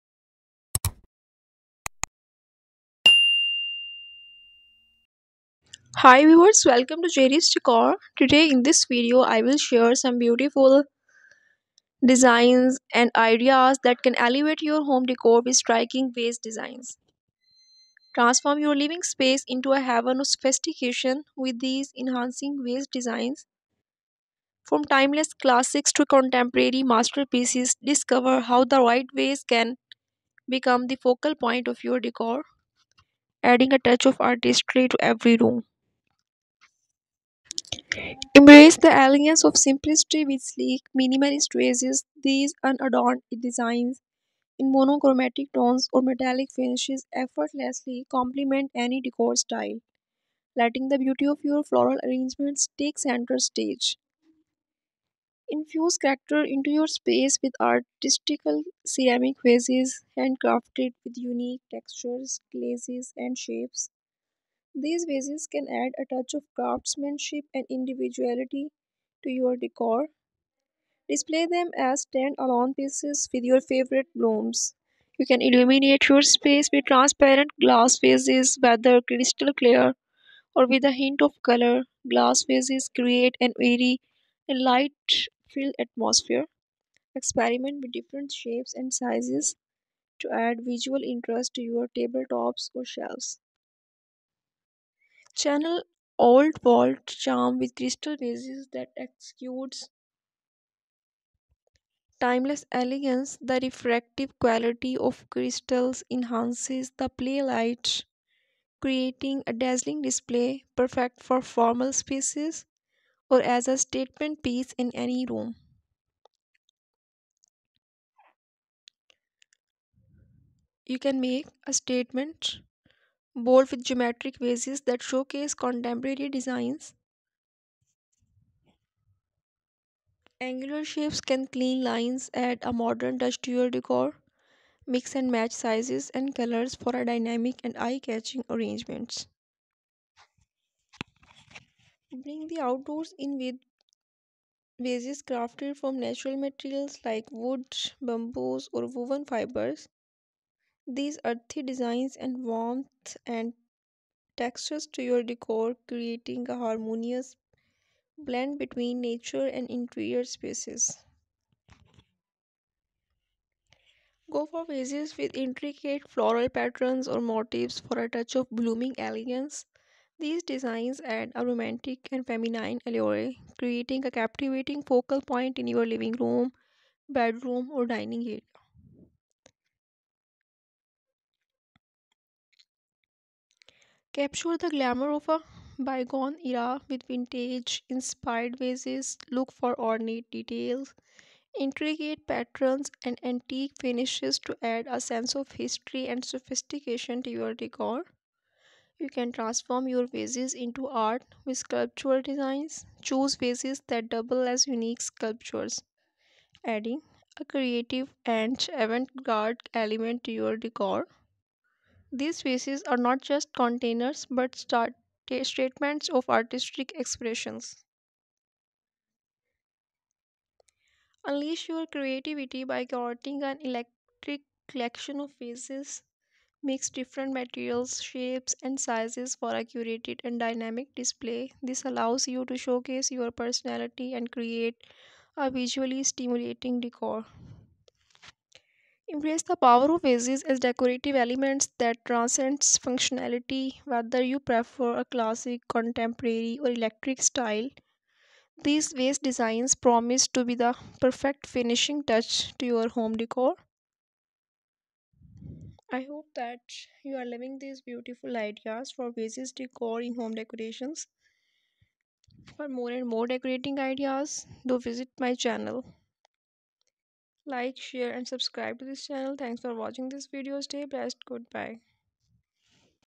Vase Designs for Stylish Interiors | Vase Decor for Modern Living | Home Decor Ideas | Jaeri's Decor
Indulge in the allure of modern living with Jaeri's Decor as we unveil a curated collection of Vase Designs for Stylish Interiors. Elevate your home decor with these artistic and contemporary vases, each a statement piece crafted to infuse your space with sophistication and visual appeal.